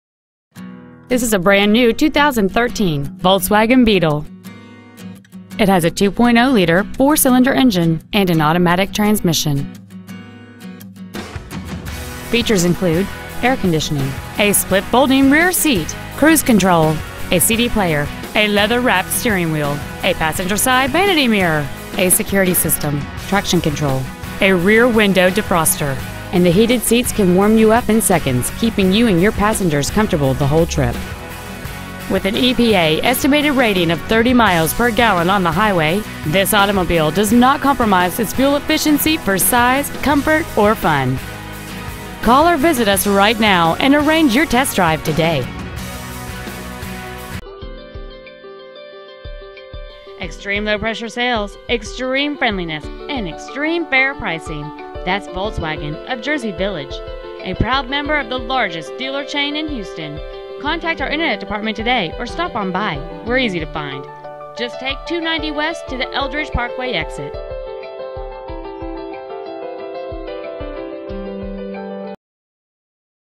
2013 Volkswagen Beetle Houston TX 77065
Year : 2013
 Make : Volkswagen
 Model : Beetle
 Engine : 2.0L I-4 cyl
 Trans . : Automatic
 Exterior : Candy White

 Interior : Black

 2013 Volkswagen Beetle Houston TX